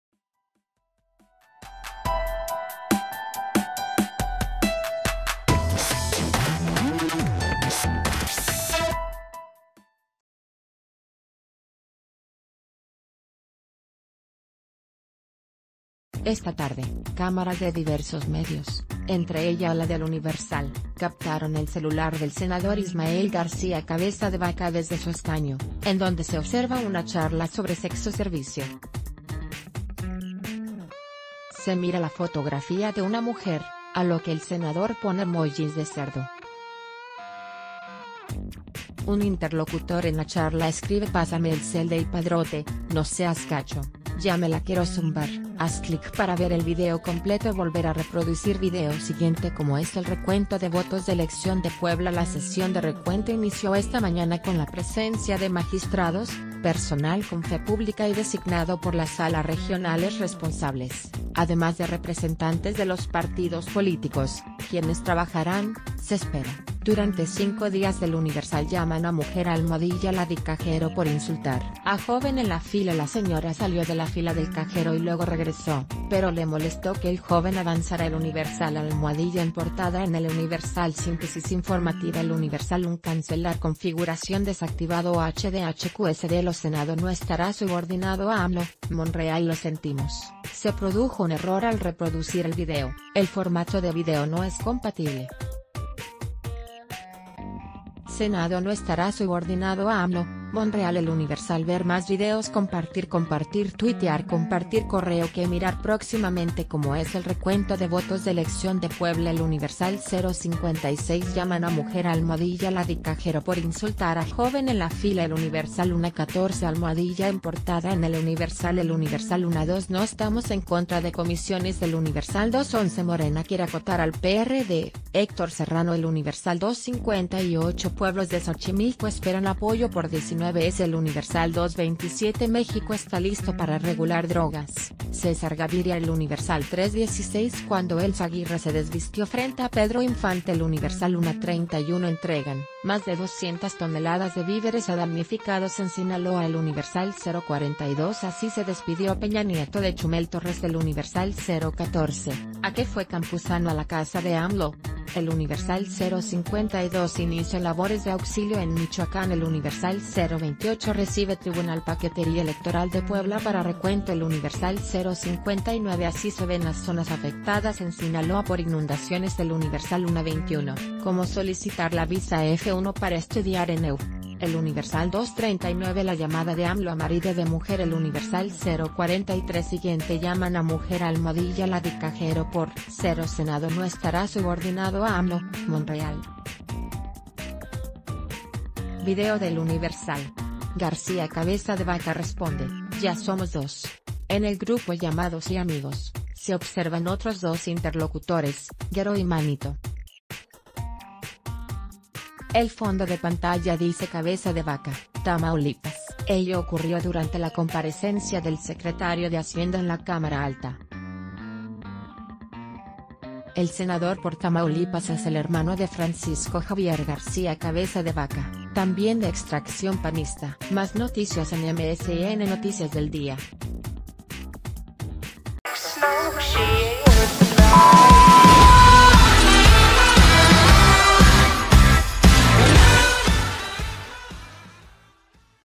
Senador chatea sobre sexoservicio en su escaño
Esta tarde, cámaras de diversos medios, entre ellas la de EL UNIVERSAL, captaron el celular del senador Ismael García Cabeza de Vaca desde su escaño, en donde se observa una charla sobre sexoservicio. Se mira la fotografía de una mujer, a lo que el senador pone emojis de cerdo. Un interlocutor en la...